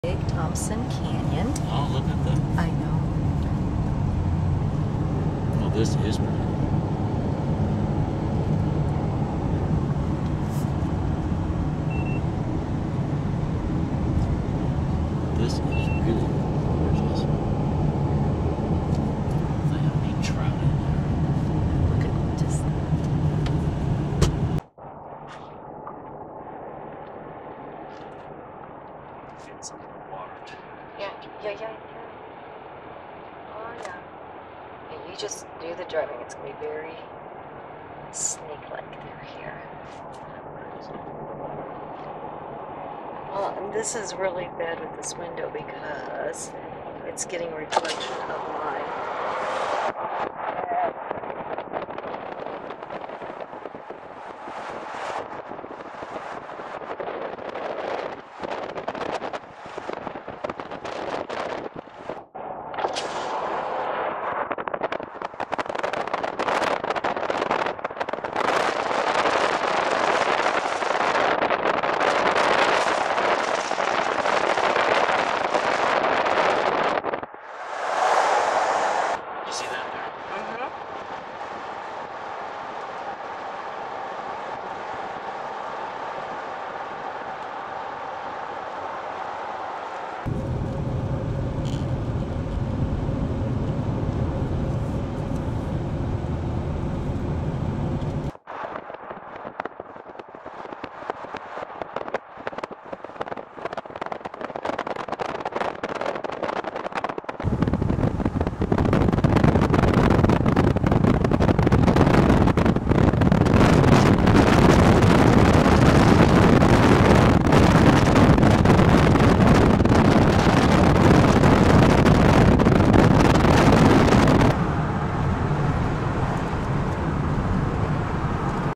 Big Thompson River viewed from U.S. 34 west between Loveland and Estes Park pre-2013 flood
This shows the Big Thompson River as it looked from U.S. 34 west in July of 2008 before the September 2013 flood. This has all changed as a result of the flood.